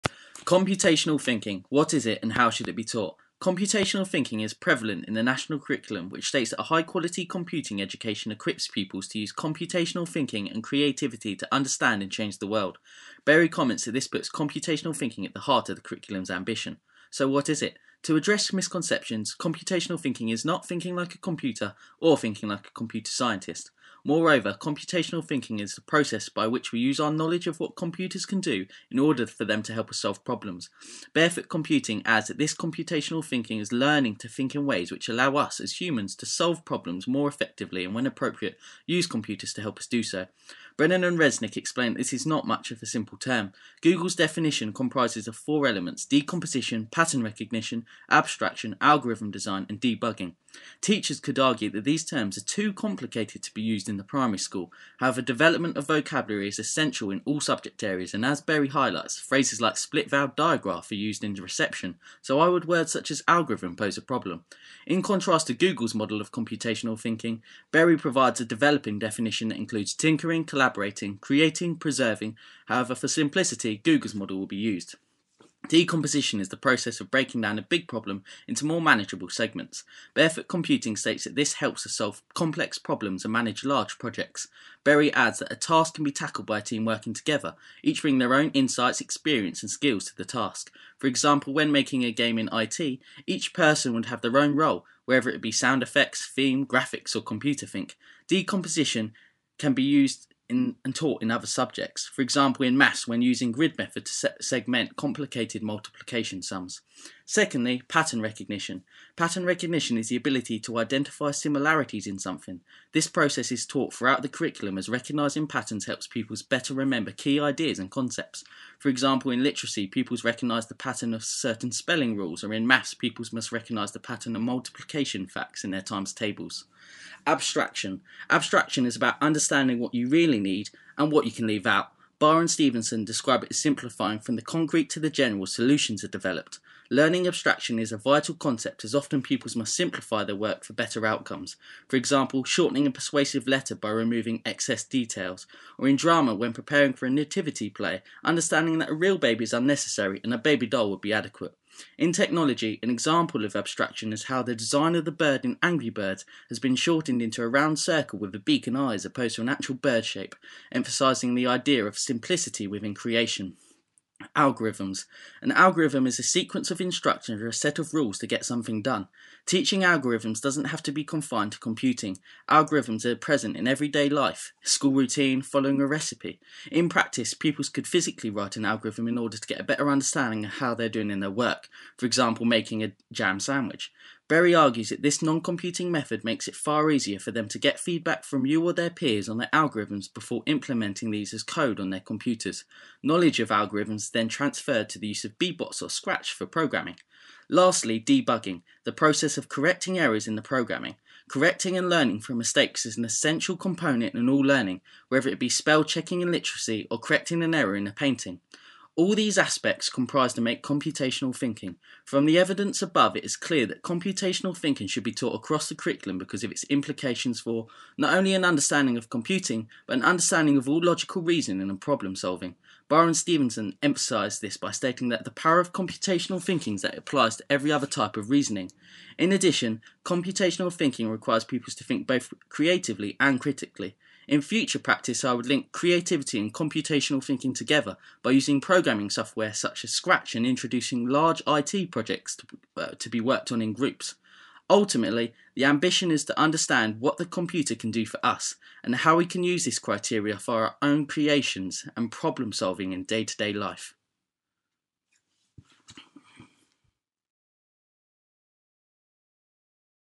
Computational Thinking: What is it? How should it be taught?
This video explains what computational thinking is, and how it can be taught in the classroom, specifically the primary school classroom. I go through the definition of computational thinking, break down its different elements (decomposition, pattern recognition, abstraction, algorithm design and debugging), and also give practical tips on how to teach computational thinking in ICT and other subjects in the National Curriculum.